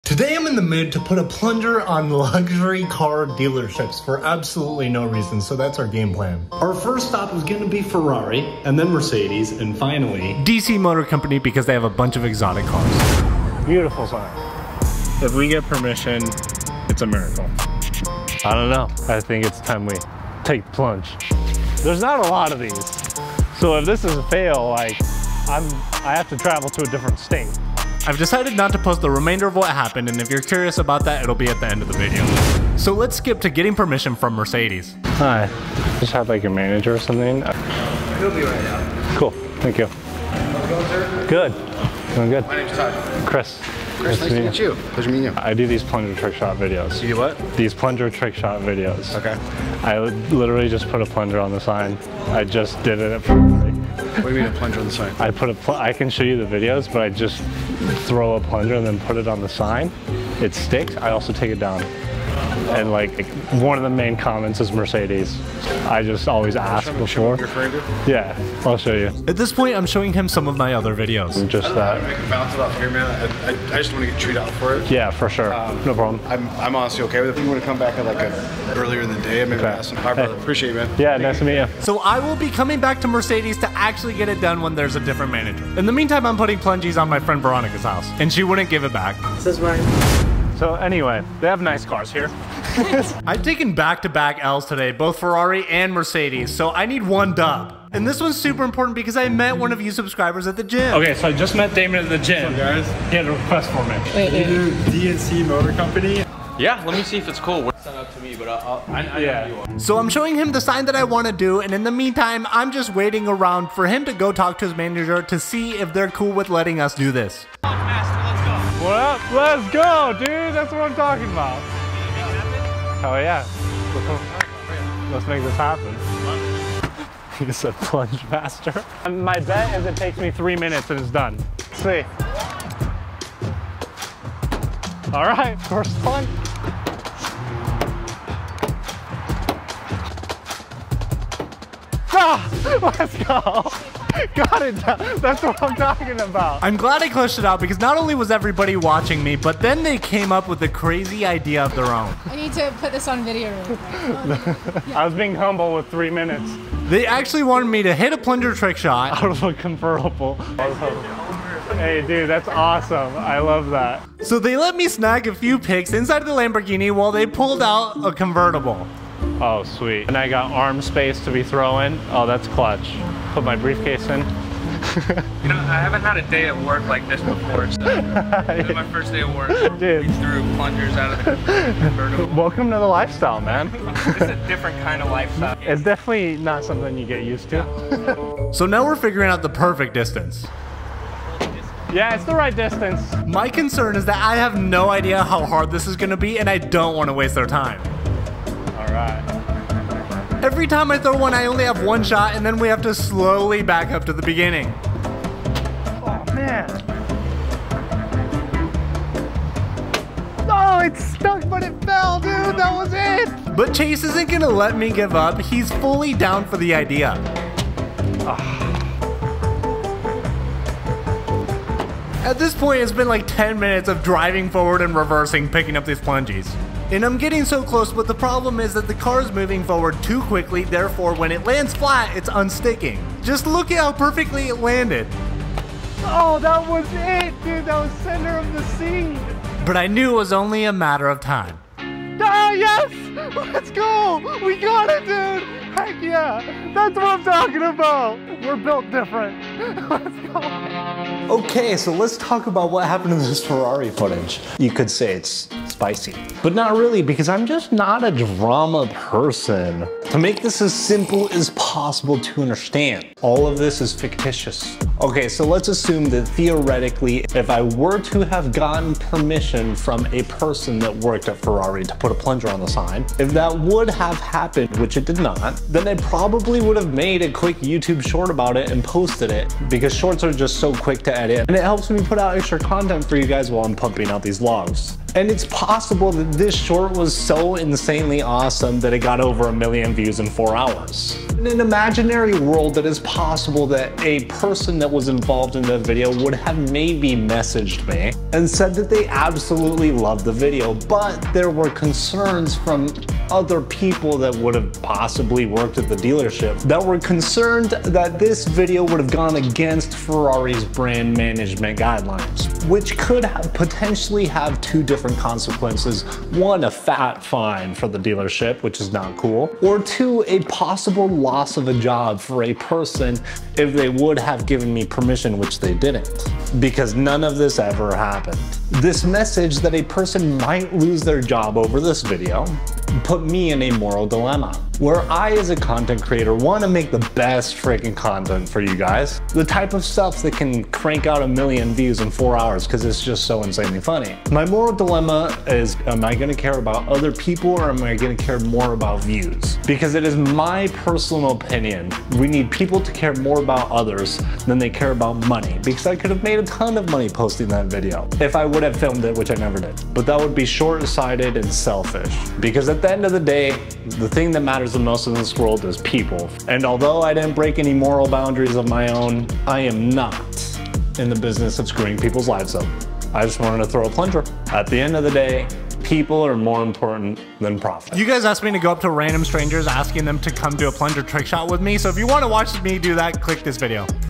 Plunger Trick Shots at Luxury Car Dealerships
I put a plunger on luxury car dealership signs.

follow these cuz youre awsome